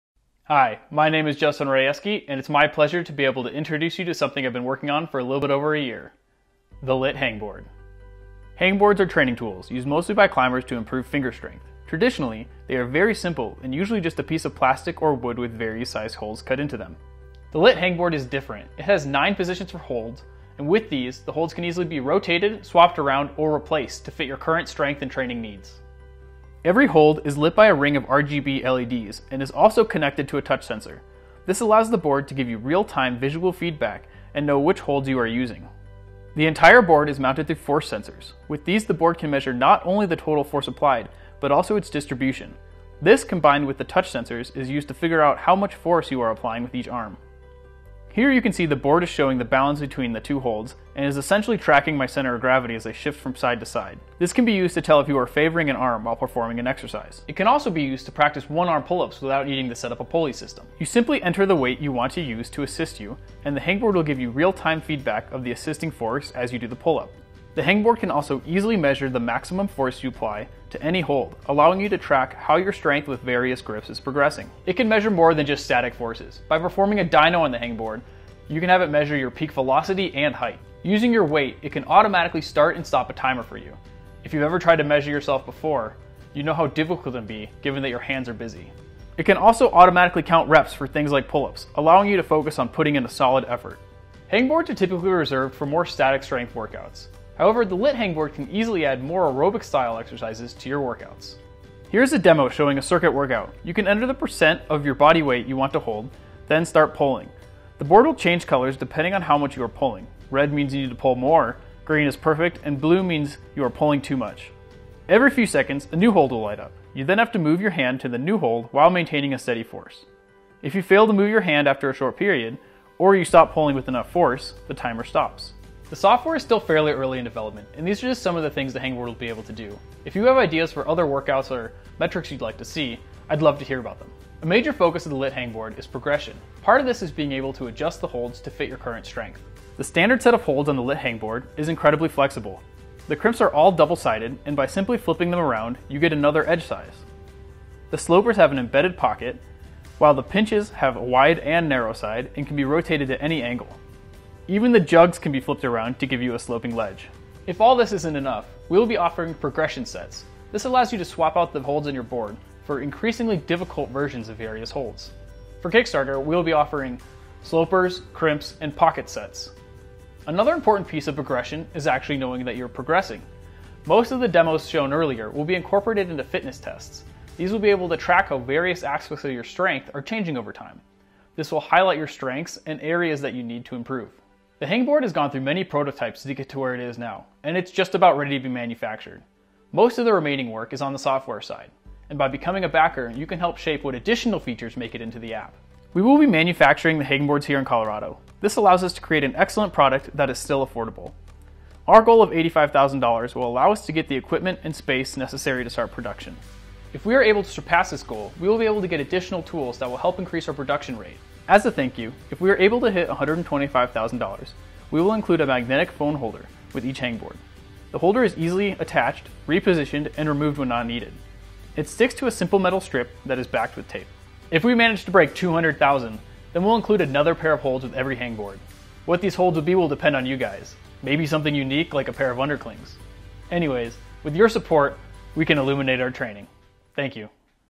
Lit Hangboard: Climbing Illuminated
Today's Featured Kickstarter!!

Lit Hangboard: Climbing Illuminated - By Justin Rajewski

Story:

I've been climbing for about five years and have slowly been getting better. But after that initial phase of massive improvements when you first begin, things start to slow down and it can be hard to know if what you are doing is actually helping improve your climbing.

Sometimes you go climbing and something feels easier than you're used to. Better conditions? Is the grading soft? Are you having a killer day? Are you actually just stronger?

I created the Lit Hangboard to provide insight into your training and improve the exercises themselves. The board is able to easily measure and track your performance metrics over time. No more spreadsheets.

The hangboard has nine holes that the holds can be bolted into. Each is surrounded by a ring of RGB LEDs and touch sensor embedded into the board.

Through these, the hangboard is able to tell what holds you are holding and give you real time visual feedback.

The entire board is mounted through load sensors that allow the hangboard to measure not only the total force applied to the board but also its distribution. All of these sensors allow the board to figure out how much force you are applying with each arm.

With these sensors, the handboard is able to figure out all kinds of useful metrics.

For example, it can tell if you are favoring an arm while hanging or performing pull ups. It can also be used to practice one-arm pull ups without having to setup a pulley system or elastic bands to assist you. You simply tell the hangboard how much assisting weight you want and use your other arm to do the assisting. The board gives you real time feedback on how much force you are "cheating" with.